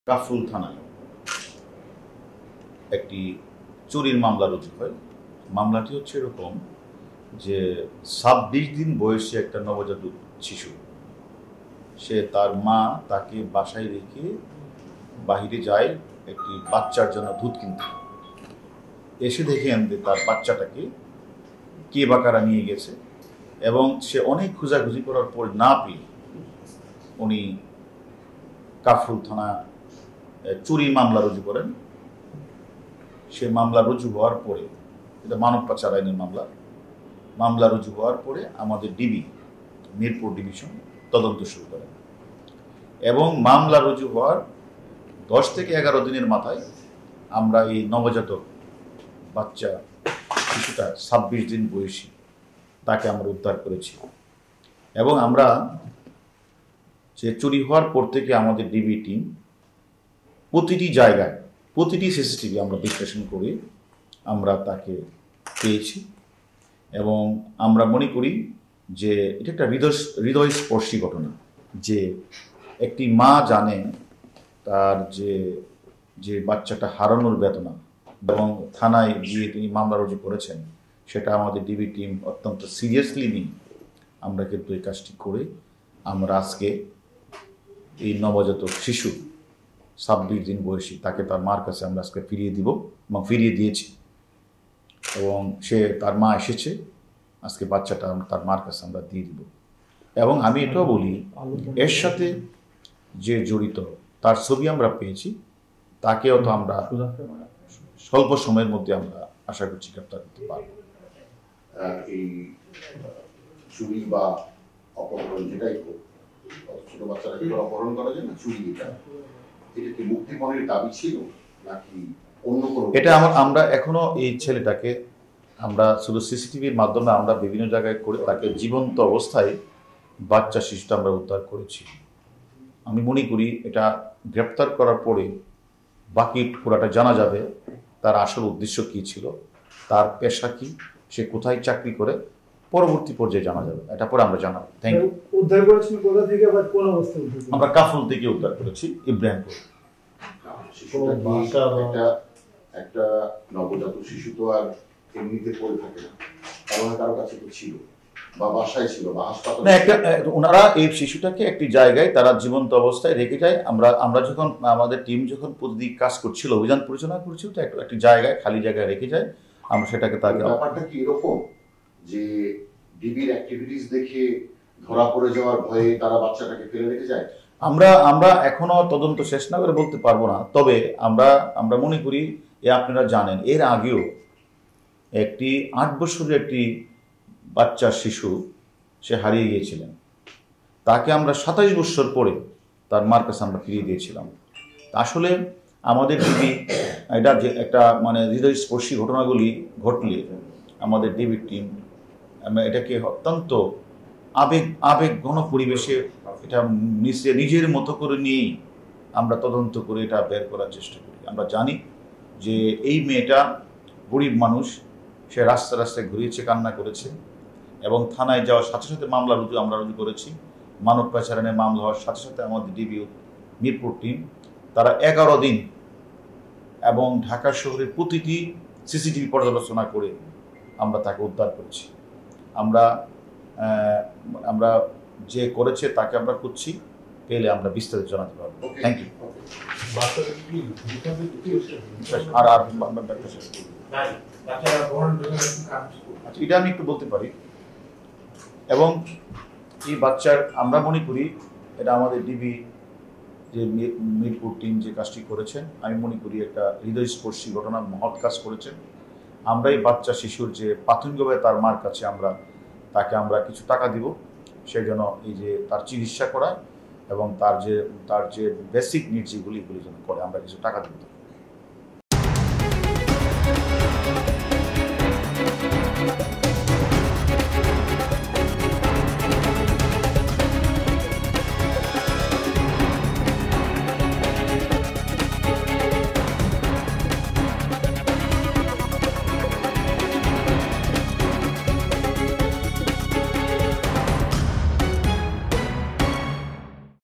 কিভাবে ধ-রা পড়ল ২৬দিন বয়সী নবজাতক শিশু চো-র | DB Police | SATV
কিভাবে ধ-রা পড়ল ২৬দিন বয়সী নবজাতক শিশু চো-র

SA TV Location:
Bangladesh